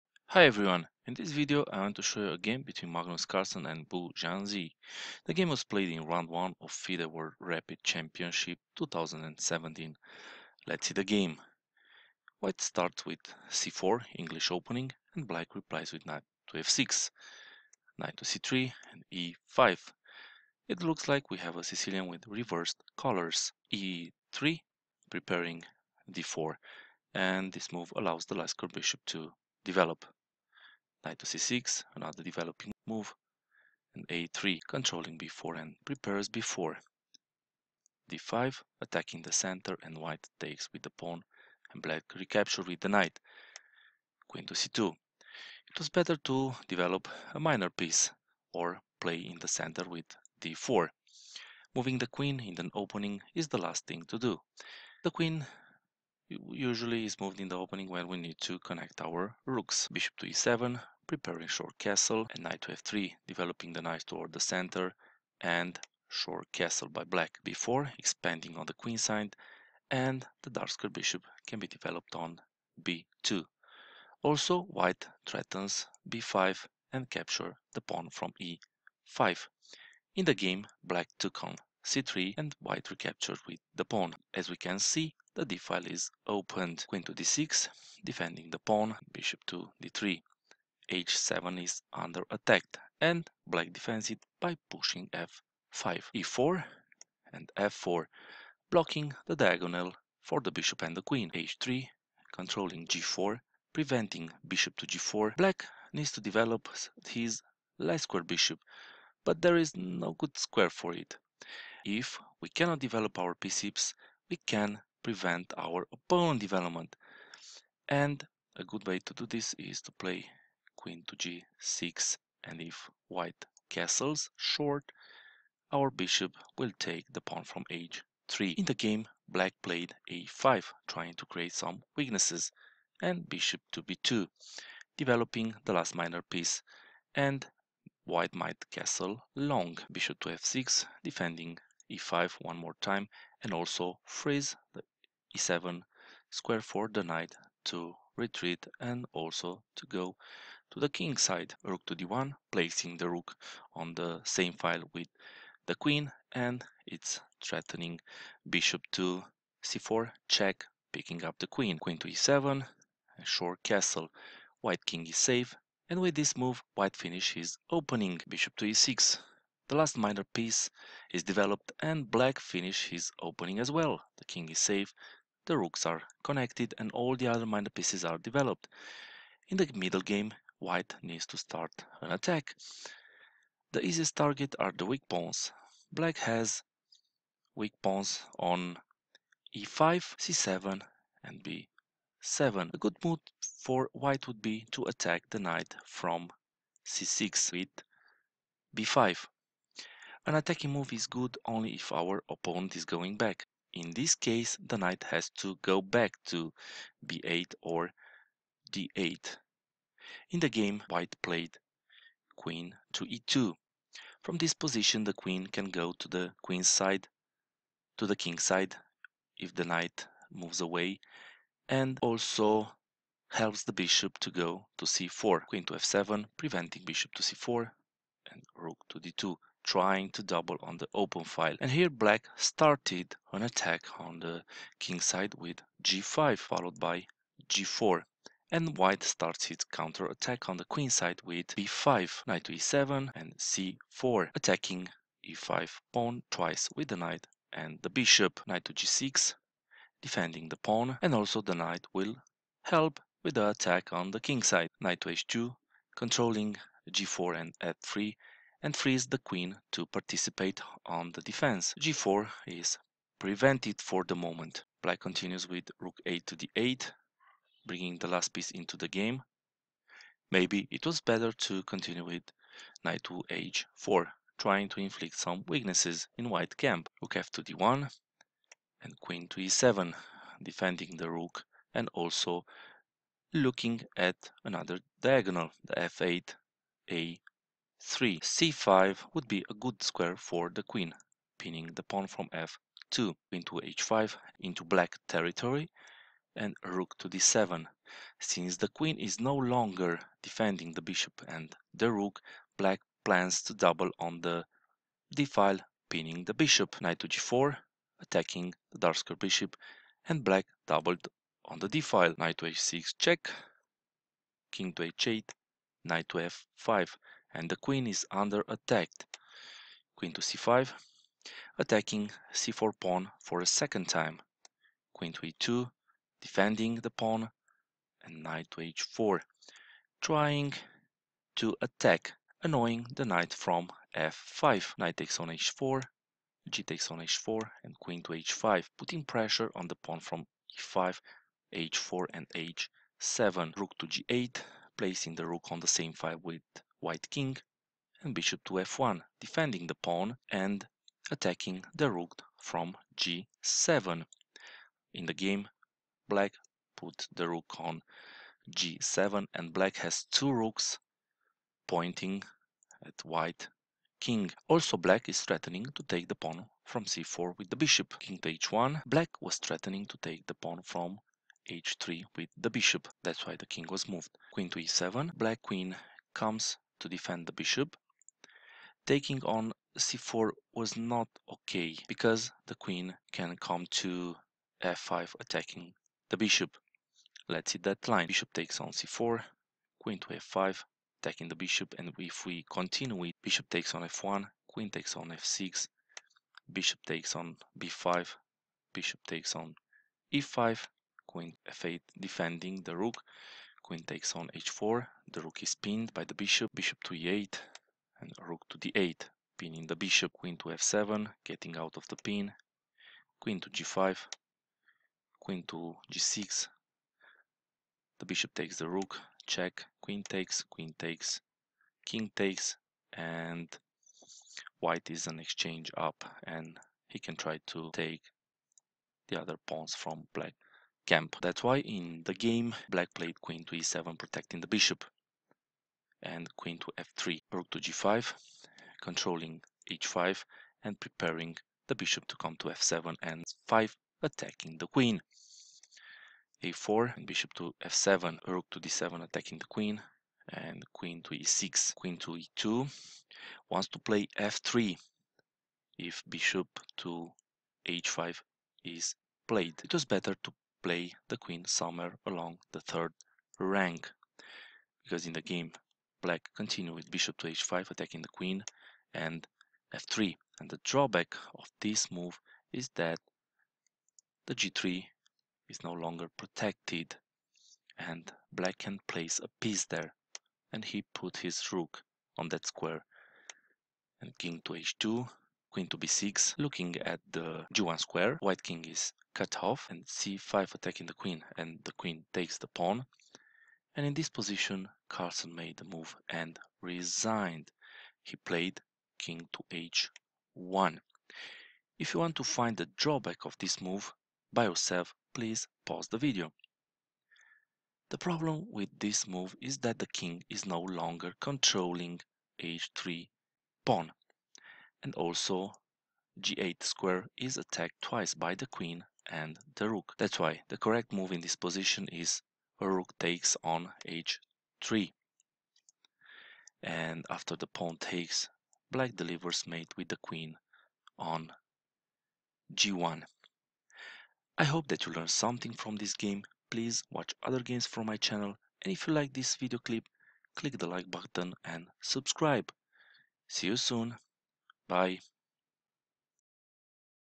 Magnus Carlsen vs Bu Xiangzhi FIDE WRC R1 - English Opening: King's English Variation
In this video I want to show you a game between Magnus Carlsen and Bu, Xiangzhi. The game was played in round 1 of FIDE World Rapid Championship.

[Event "FIDE World Rapid Championship"]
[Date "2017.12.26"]
[Round "1"]
[White "Carlsen, Magnus"]
[Black "Bu, Xiangzhi"]
[Result "0-1"]
[WhiteElo "2908"]
[BlackElo "2654"]
[Variant "Standard"]
[ECO "A22"]
[Opening "English Opening: King's English Variation, Two Knights Variation"]

1. c4 Nf6 2. Nc3 e5 { A22 English Opening: King's English Variation, Two Knights Variation } 3. e3 Nc6 4. a3 d5 5. cxd5 Nxd5 6. Qc2 Be7 7. Nf3 O-O 8. b4 Nxc3 9. dxc3 Qd6 10. Bd3 f5 11. e4 f4 12. h3 a5 13. Bb2 Bf6 14. Rd1 Qe7 15. O-O Be6 16. Qe2 Qf7 17. Rd2 g5 18. b5 Ne7 19. c4 Ng6 20. Nh2 Rad8 21. Rfd1 Qe7 22. Qh5 Rd7 23. Ng4 Rfd8 24. Nh6+ Kh8 25. Nf5 Qc5 26. Qe2 Nh4 27. Nxh4 gxh4 28. Qh5 Rg8 29. Bf1 Rdg7 30. Kh1 Qe7 31. Qf3 Rg5 32. a4 Bf7 33. Rd7 Qe6 34. Qe2 Bh5 35. f3 Rg3 36. Kh2 Qb6 37. c5 Qxc5 38. Kh1 { White resigns. } 0-1